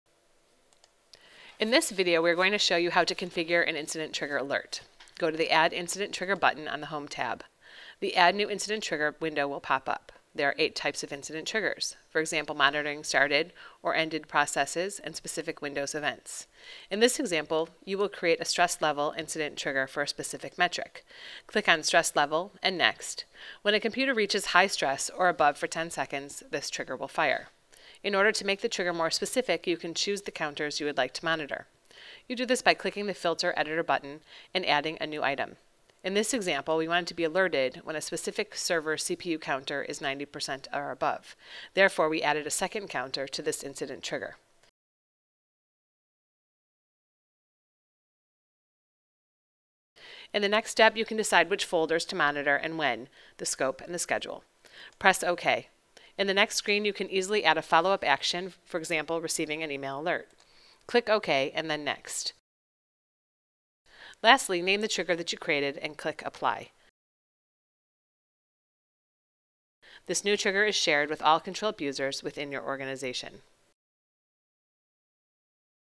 Getting Started: Triggers
Learn how to create incident trigger alerts within the ControlUp Real-time console.